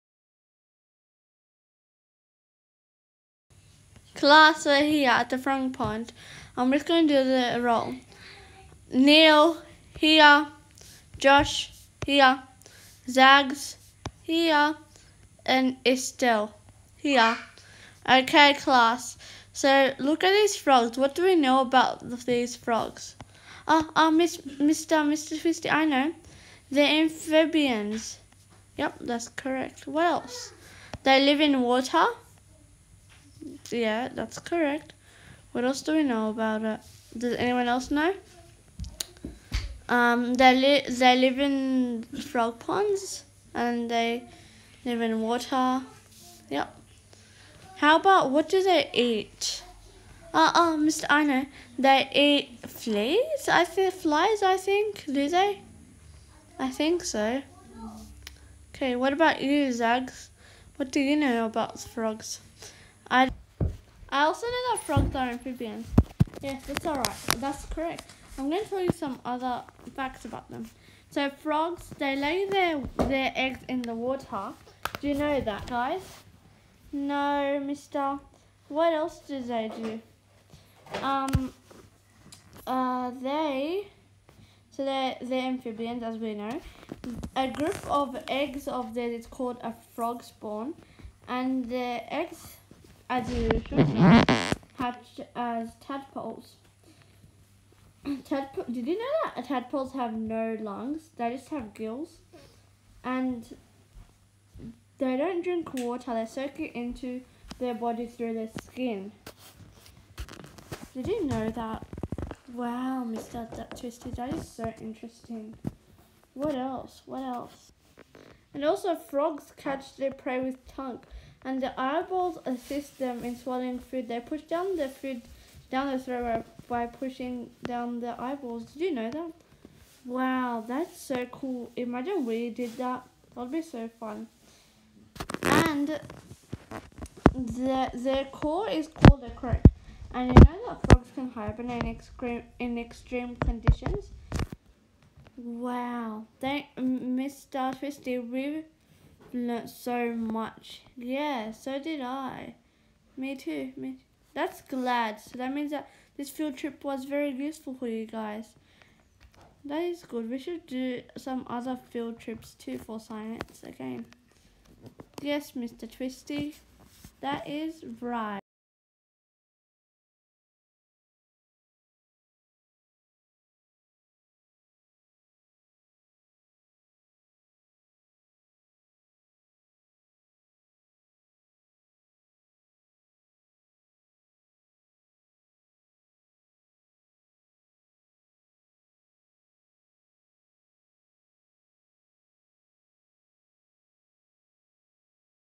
What are frogs
The class goes to a field trip at the frog pond and learn interesting facts about them